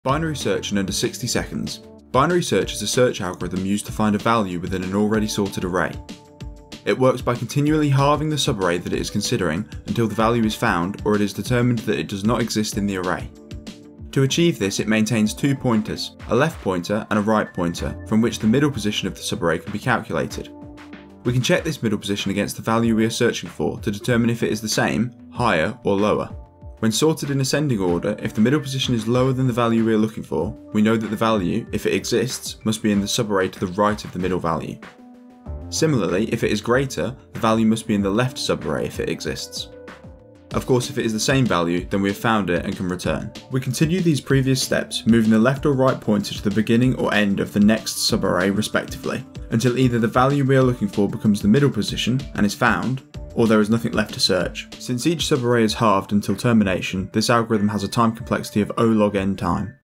Binary Search in Under 60 Seconds
In this video, I will explain the Binary Search algorithm in under 60 seconds. More accurately, in exactly 59 seconds.